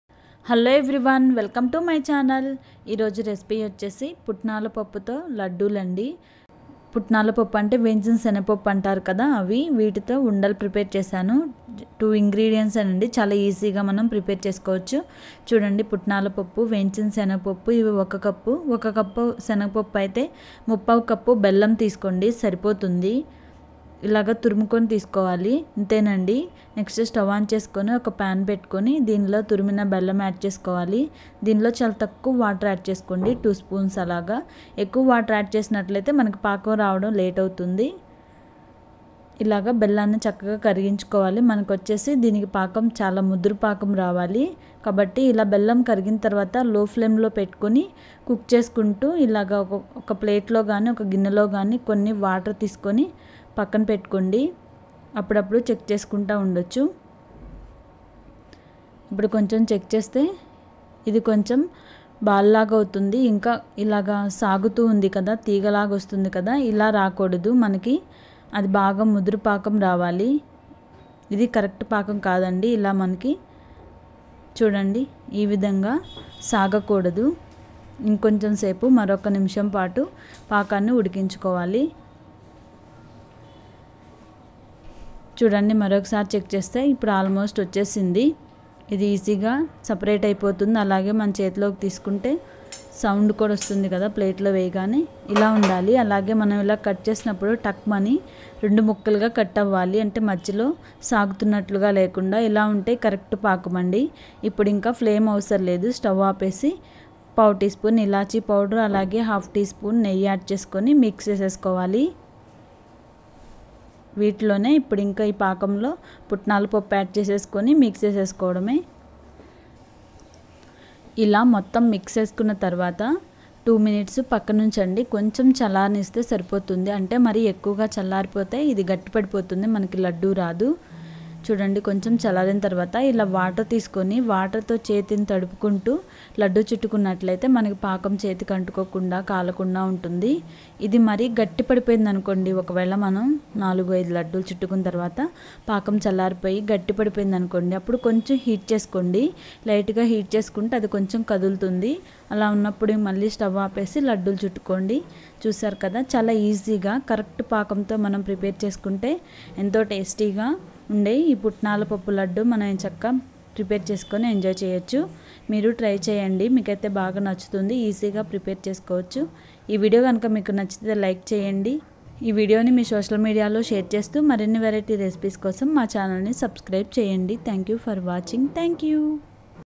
పప్పుఉండలు బాగా రావాలంటే పాకం ఇలా చేయాలి-Pappu Undalu Recipe with Tips-Putnala Laddu in Telugu-Ladoo
ingredients
1.Fried chenna dal(Putnala pappu)-1cup
2.Jaggery-3/4cup
3.cardamom powder-1/4tsp
4.ghee-1/2tsp